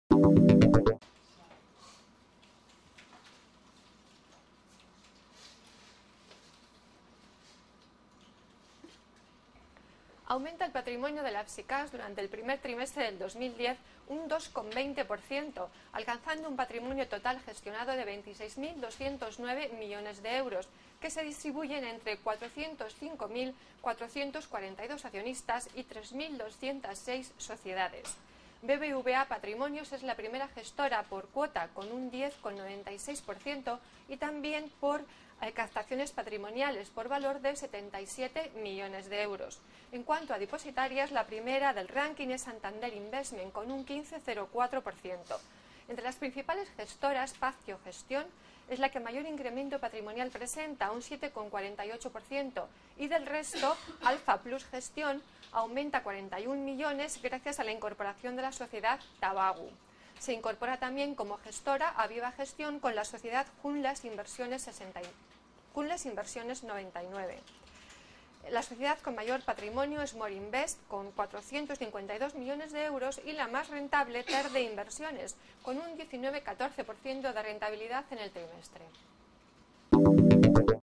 VDOS: el patrimonio de las SICAV creció el 2,20% hasta marzo
Madrid, 24 jun (EFE).- (Imagen: Ángel Herrera).- El patrimonio gestionado por las Sociedades de Inversión de Capital Variable (SICAV) creció el 2,20 por ciento en el primer trimestre de 2010 en comparación interanual, hasta alcanzar 26.209 millones de euros, distribuidos entre 405.442 accionistas.

Así lo explicó en declaraciones a EFE la directora de análisis de VDOS, Paula Mercado, que añadió que BBVA Patrimonios se mantiene en primera posición, tanto por gestoras, con una cuota de 10,96 por ciento, como por captaciones patrimoniales netas, con 77 millones de euros.